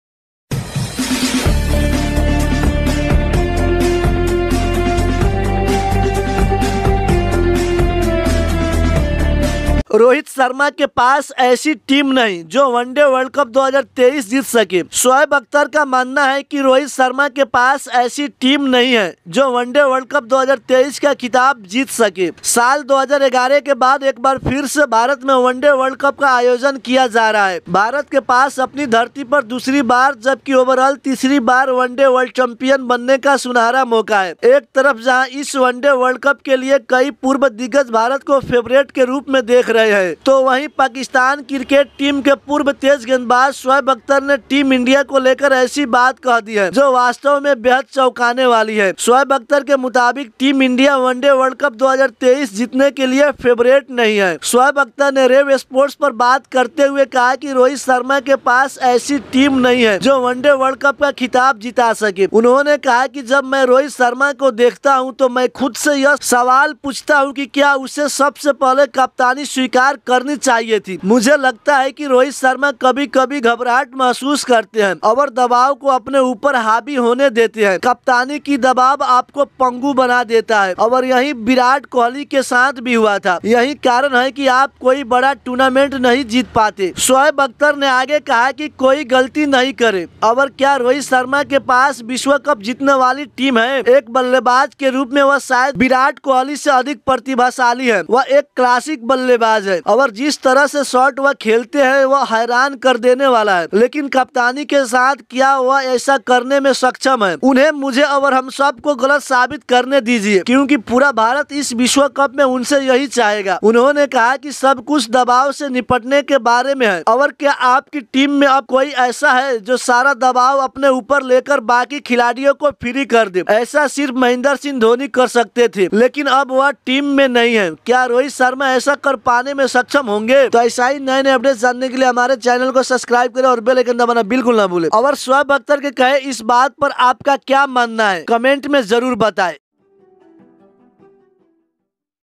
टीम इंडिया के पास वॉर्ल्ड कप 2023 का खिताब जीता ने वाले खिलाडी नही है,शोऐब अख्तर का मानना है रोहित..
टीम इंडिया के पास वॉर्ल्ड कप 2023 का खिताब जीता ने वाले खिलाडी नही है,शोऐब अख्तर का मानना है रोहित शर्मा के पास ऐसा खिलाडी नही, जो वॉर्ल्ड कप जीता सके